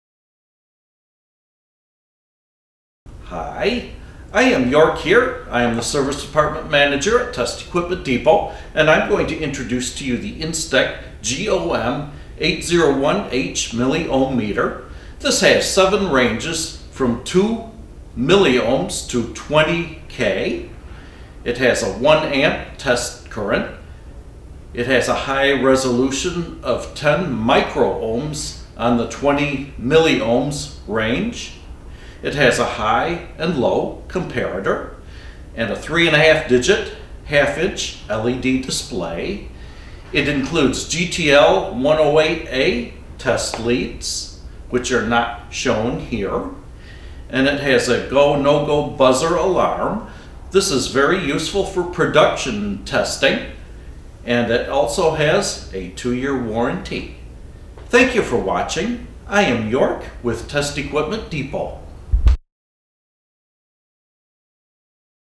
Instek GOM-801H Milli-Ohm Meter
The Instek GOM801H Milliohm Meter is designed for simplicity in measuring the contact resistance of switches, relays, plugs, connectors, etc.

-7 Ranges from 20 mΩ  to 20 kΩ
-1 Amp Test Current
-High-Resolution of 10 µΩ on the 20 mΩ range.
-Hi/Lo Comparator
-3 ½ Digit - 0.5" LED Display
-Includes GTL-108A Test Leads
-Go / No Go Buzzer Alarm
-2 Year Warranty